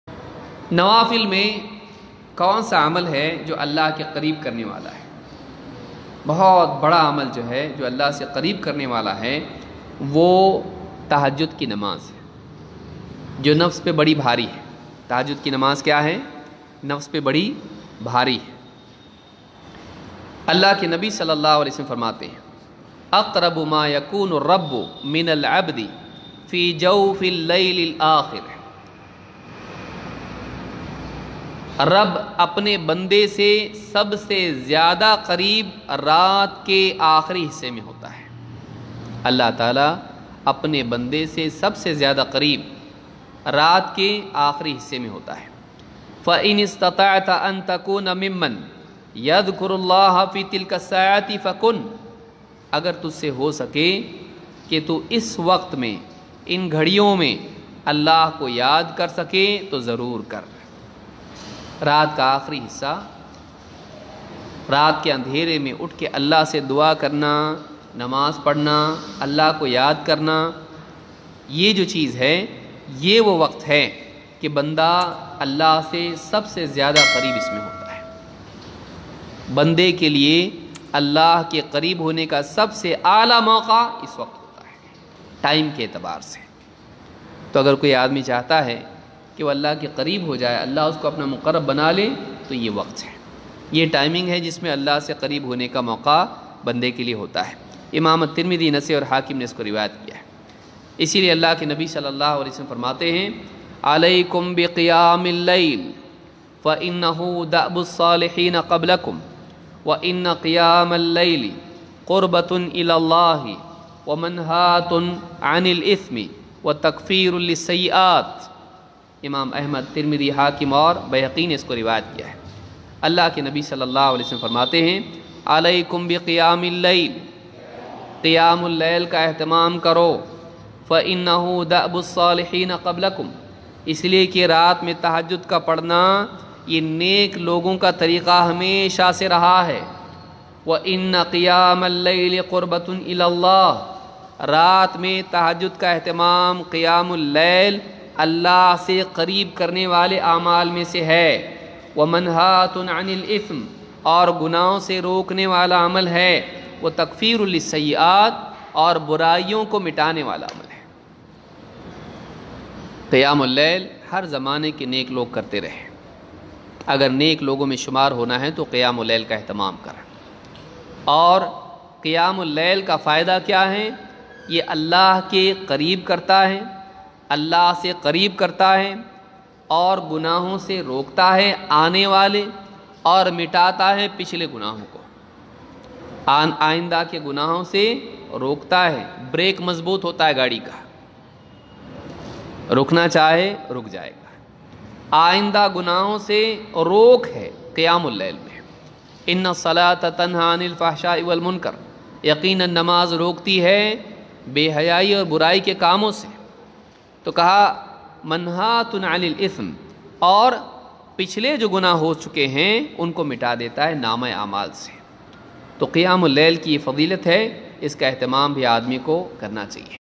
Kaun sa Nafil amaal jo Allah se nazdeek karne wala hai | Shaykh Abu Zaid Zameer Hafidhahullah
This video clips is taken from ALLAH Se QAREEB Karne Wale A'AMAAL by Shaykh Abu Zaid Zameer (Hafidhahullah)